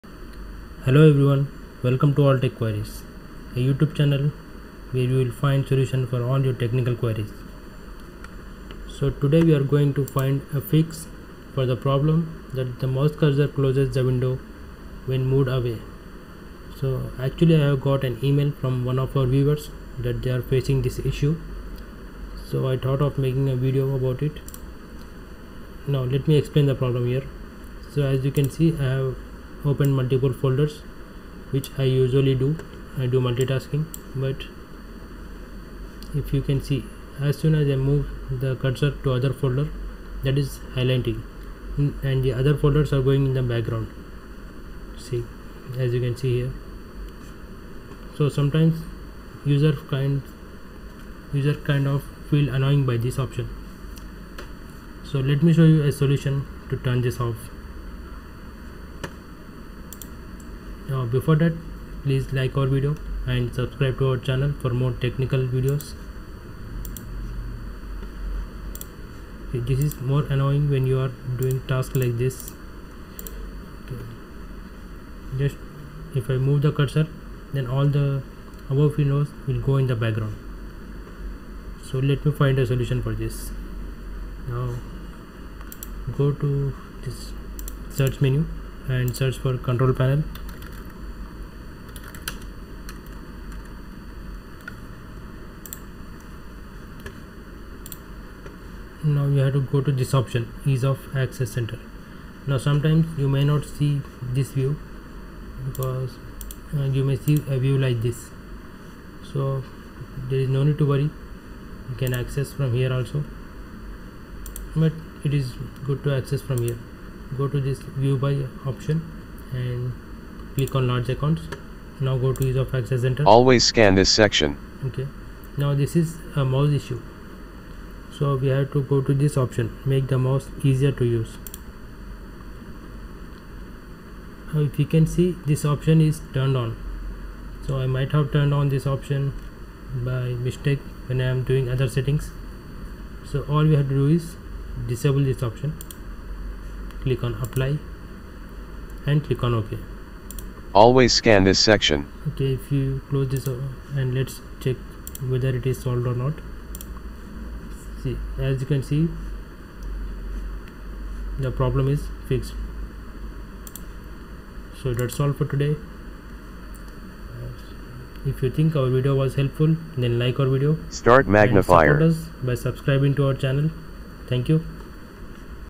[Fix] Cursor Closes Window when Moved in Windows 10
Welcome to All Tech Queries Channel (Solutions for all your Technical Queries)

About the Video:
Are you facing the issue where you move the mouse on a Window and the other Windows automatically Minimize to Taskbar? Does your Cursor Closes Window when Moved to another Window or Folder? When you move your cursor away from a Window, it auto minimizes? Then this guide will help you solve the issue in just a few minutes.

Queries Solved:
Cursor Closes Window when Moved